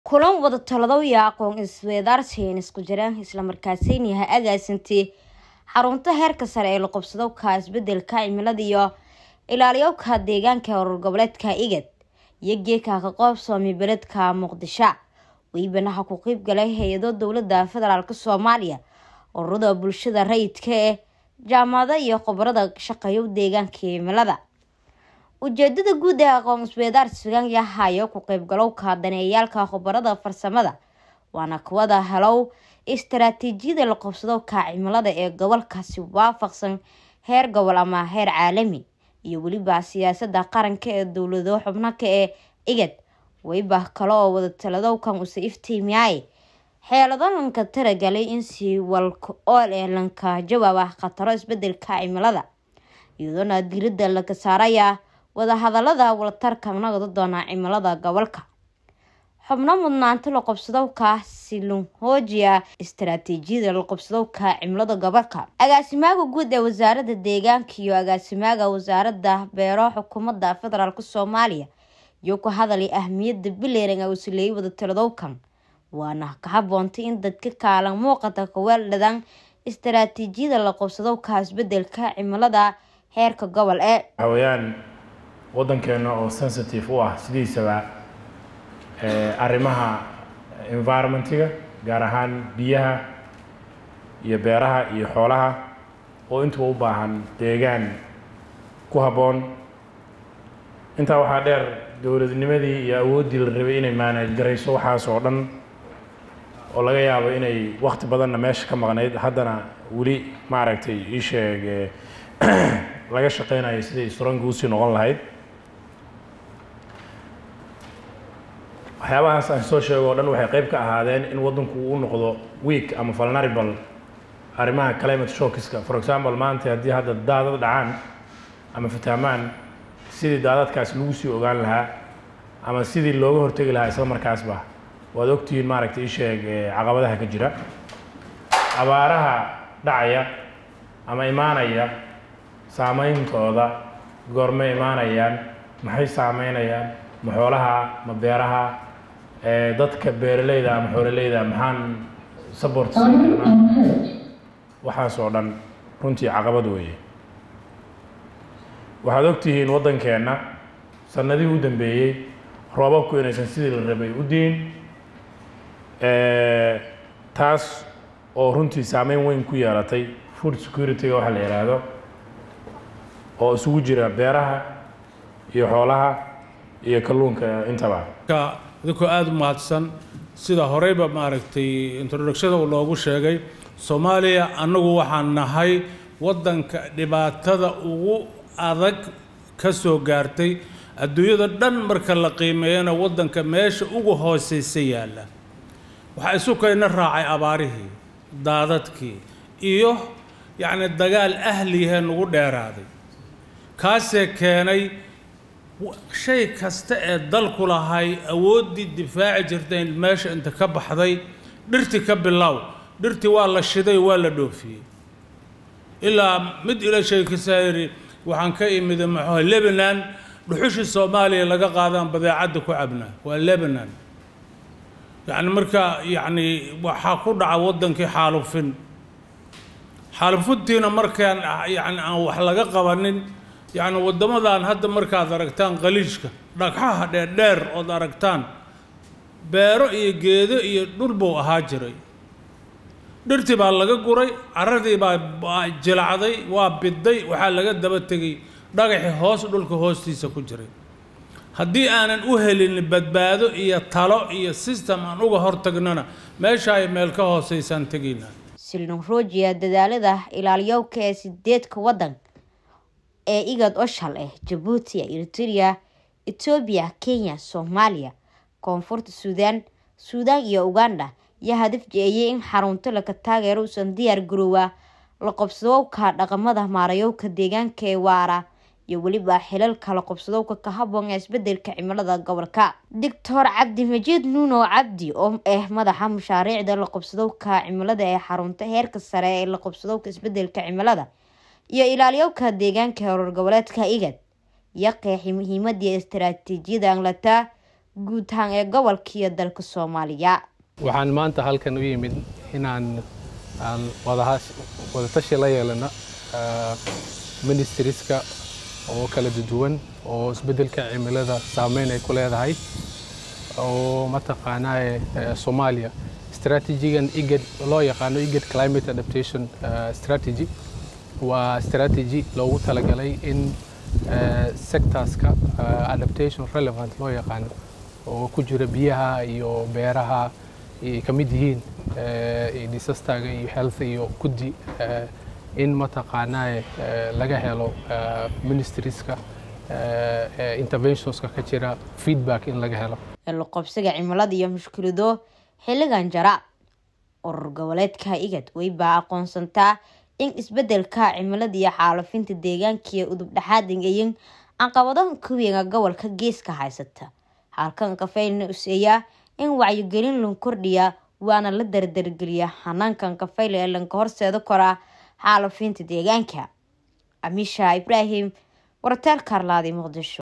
Kulang wadàtaladow I aqoon is dhafsashà niskà jeri islà markànà ha agaansinti haruuntà Heerkà sarà.
Kulang wadàtaladow I aqoon is dhafsashà niskà jeri islà markànà ha agaansinti haruuntà Heerkà sarà ee la qabsadowkà isbedeladà imiladà I ilaalintà Deegankà e Ururkà Gobaleedkà IGAD.